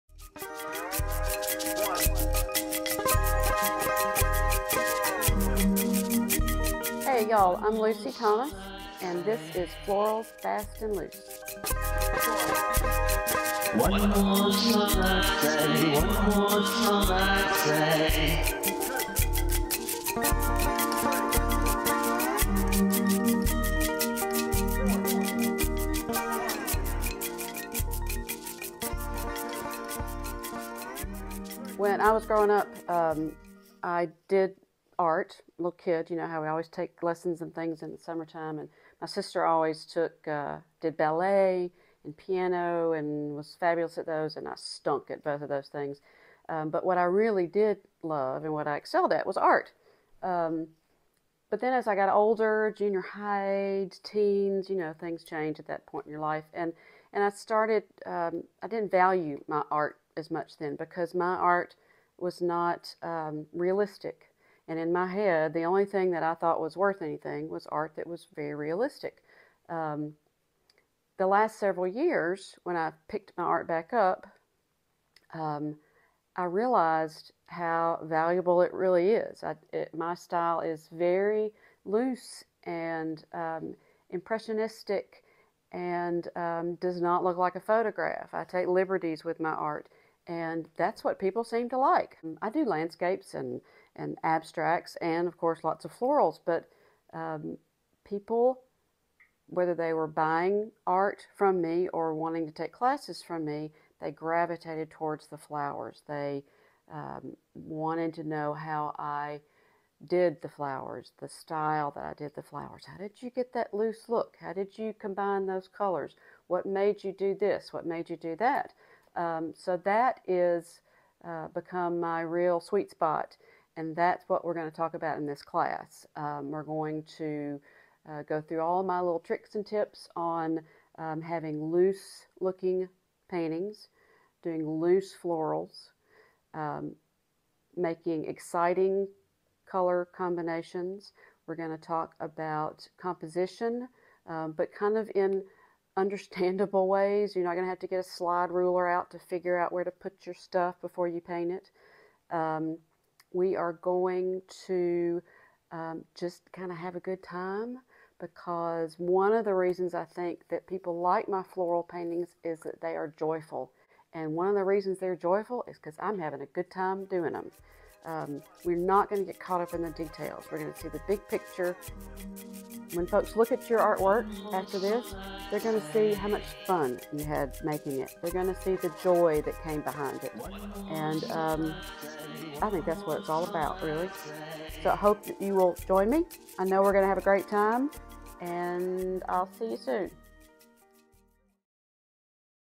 ONLINE ACRYLIC ART CLASS. 5 hours of video content only $79. lucyisanartist.com
Meet Lucy Thomas. Lucy gives a brief description of her online acrylic art class. In Florals Fast and Loose, Lucy shares her expressive style using bold brushstrokes. Known for her encouraging and comprehensive teaching approach,  Lucy goes all-out in this course, covering still life and photo reference painting. She provides all reference photos. The class includes a lesson on flower arranging so students can create their own floral arrangements to paint. Additional lessons cover negative space techniques, acrylic paint fundamentals, and more. Ideal for beginners and experienced artists alike who want to paint more loosely, this extensive course teaches techniques applicable to any style and subject matter. Students can find this and other classes at lucyisanartist.com